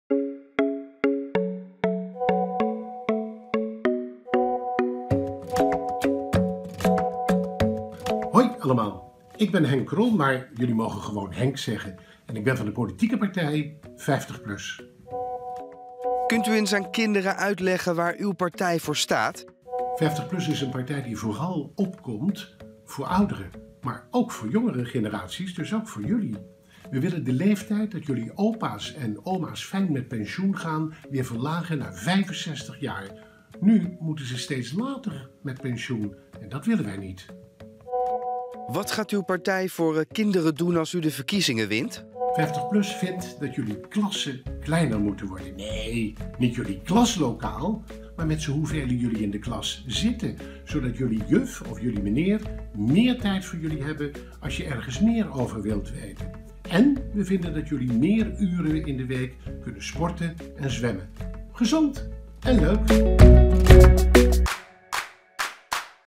Appen met Henk Krol (50PLUS)
Wij appen deze week met de lijsttrekkers van de grootste politieke partijen om ze beter te leren kennen. Op 15 maart doen deze partijen mee aan de verkiezingen voor de Tweede Kamer.

In dit gesprek vertelt Henk Krol van de partij 50PLUS meer over zichzelf en zijn partij.